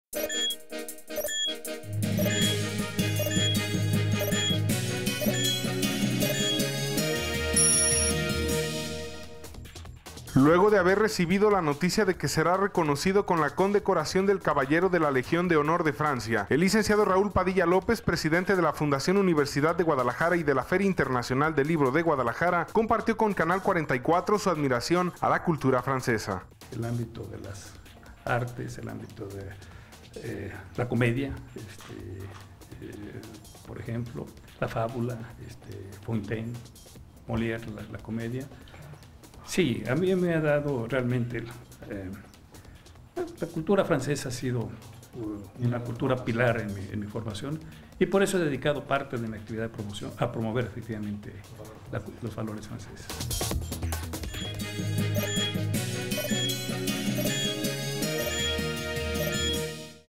Corte 44 - Raúl Padilla comparte su admiración por la cultura francesa - Junio 29 de 2012
Luego de haber recibido la noticia de que será reconocido con la condecoración del caballero de la Legión de Honor de Francia, el licenciado Raúl Padilla López, presidente de la fundación de la Universidad de Guadalajara y de la Feria Internacional del Libro en Guadalajara (FIL), compartió con canal 44 su admiración a la cultura francesa.